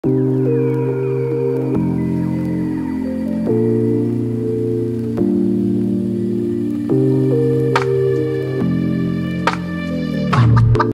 A-Log. Amsterdam, Netherlands. Vondelpark. Panoramic view of the city in summer in cloudy weather, A
A-Log. Amsterdam, Netherlands. Vondelpark. Panoramic view of the city in summer in cloudy weather, Aerial View